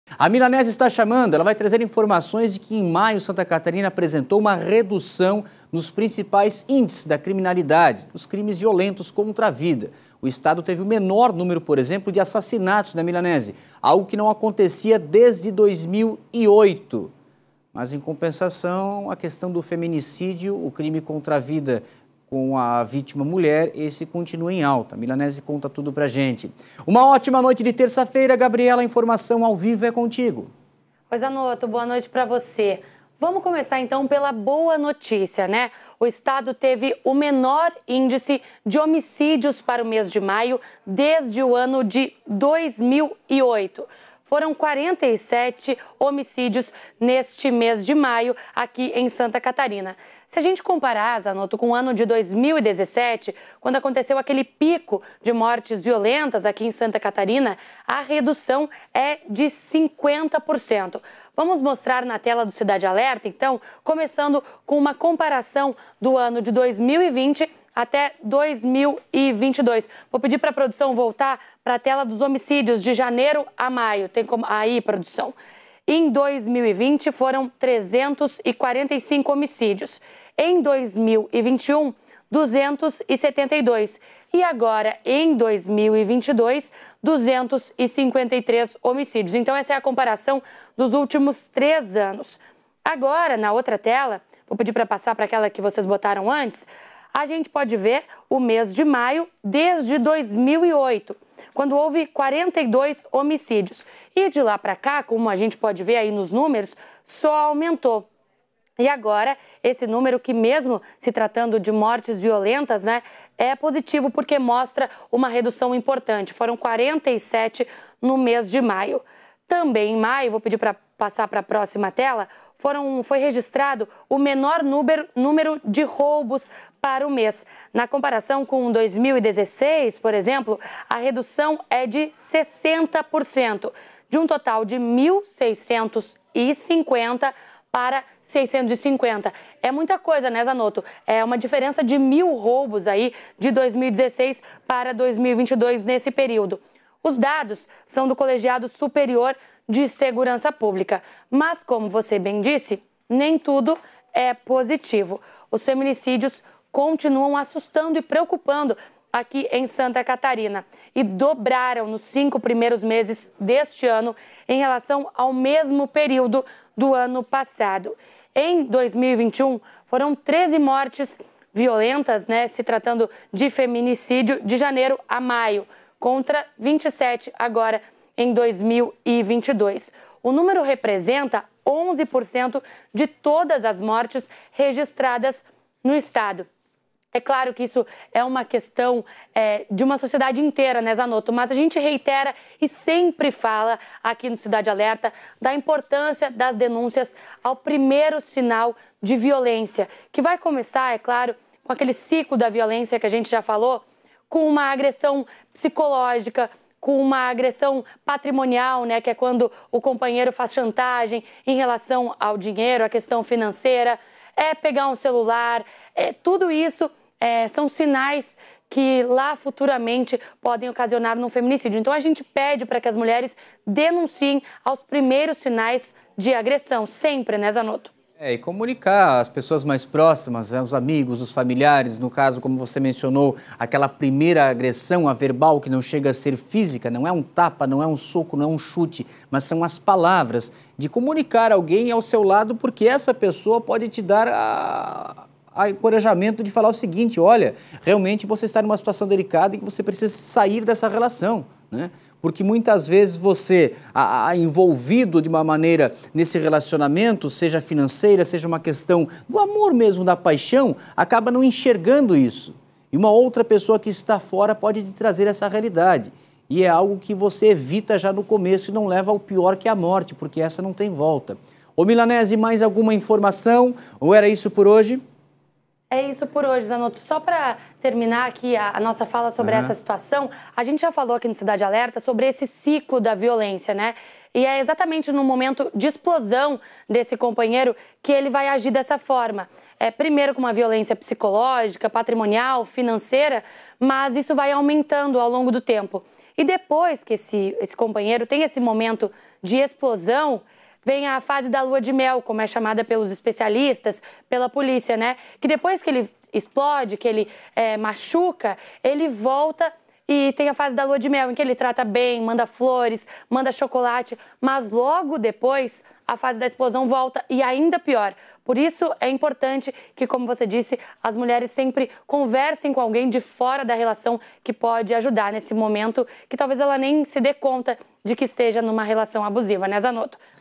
Santa Catarina tem queda nos homicídios mas crescimento nos casos de feminício no mês de maio
Em maio de 2022, o estado de Santa Catarina registrou o menor número de homicídios desde 2008. Por outro lado, dobra o número de feminicídios.

Você encontra todas as matérias do Cidade Alerta SC na área especial do portal ndmais.com.br para os conteúdos da NDTV